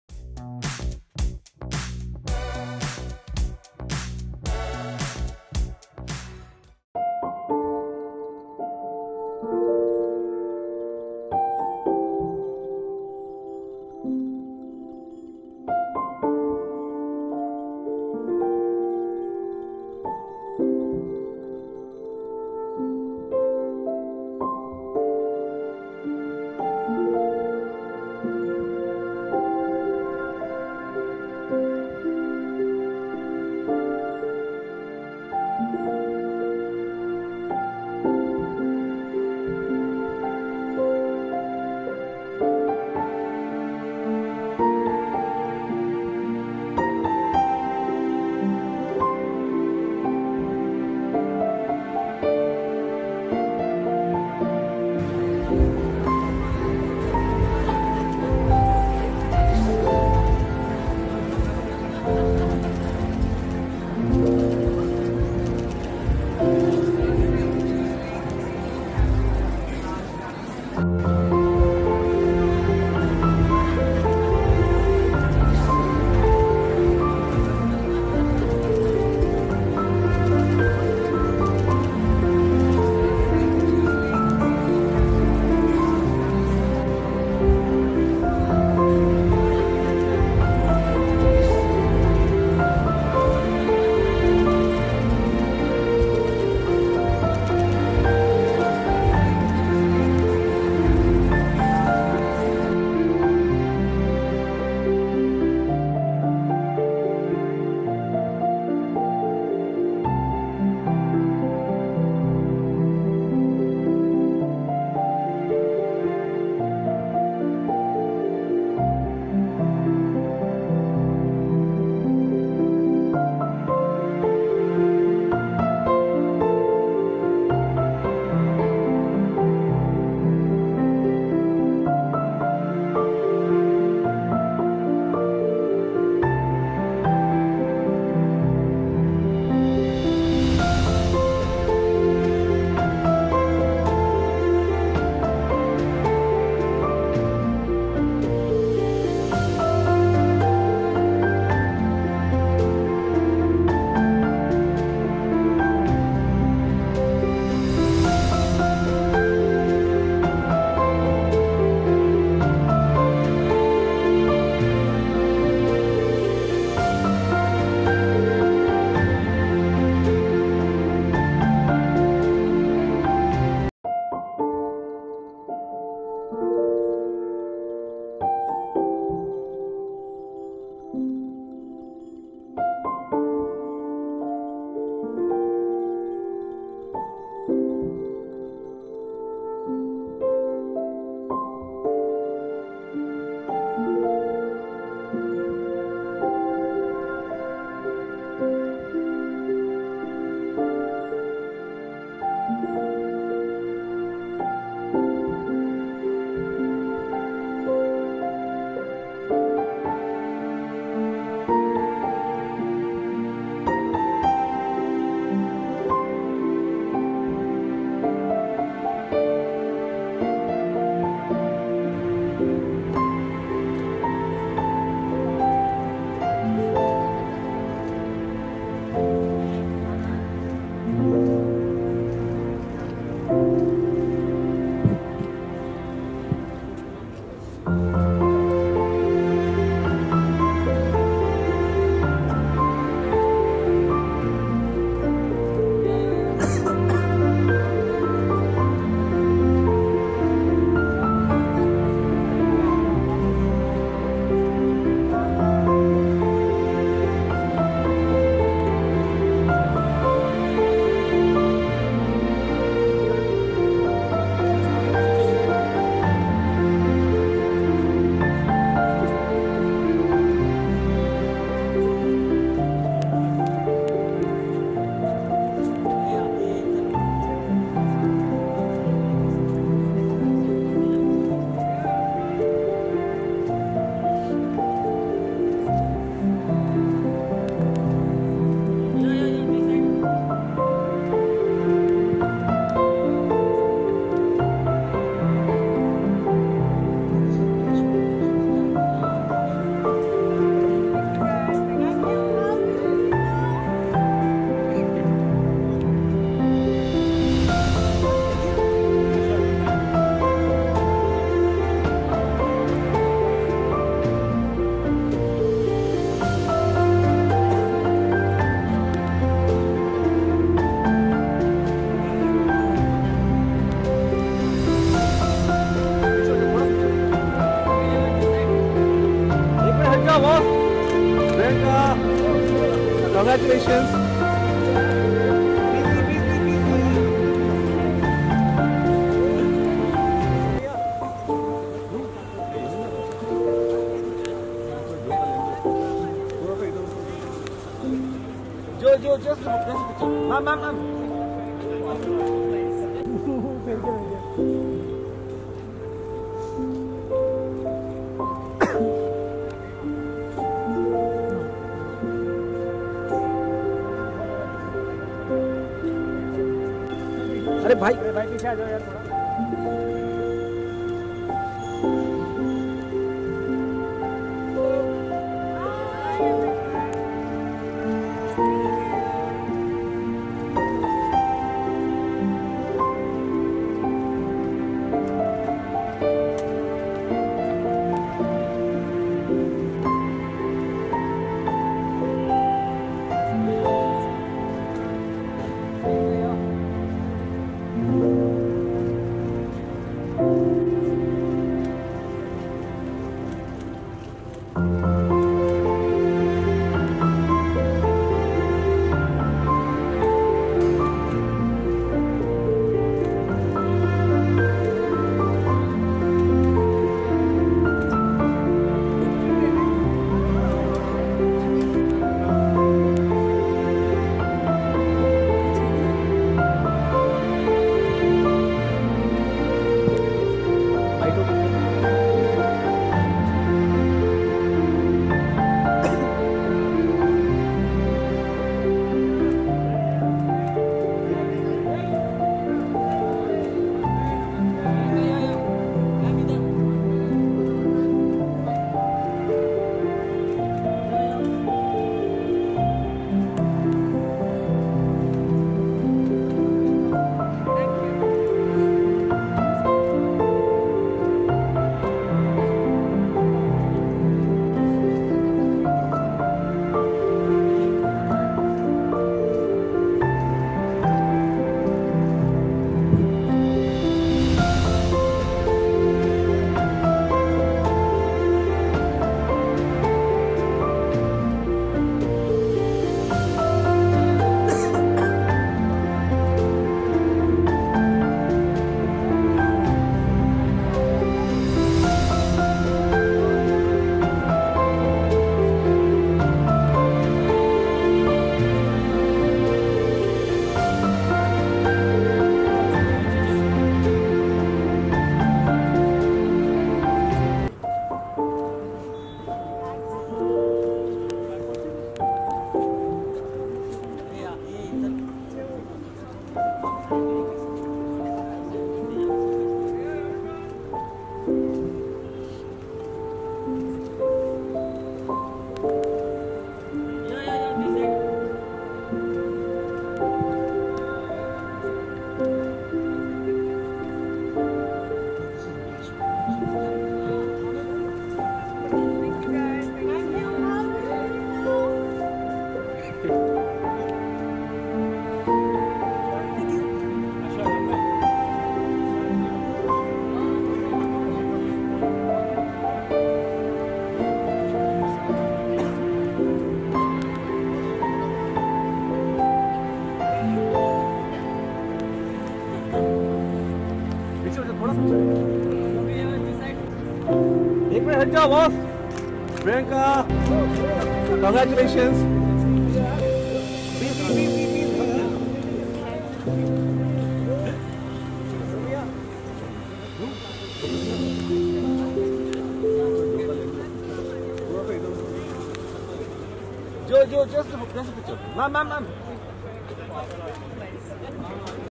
Priyanka Chopra & Nick Jonas Fairy Tale ENTRY At Royal WEDDING Reception In Delhi
Stay Updated!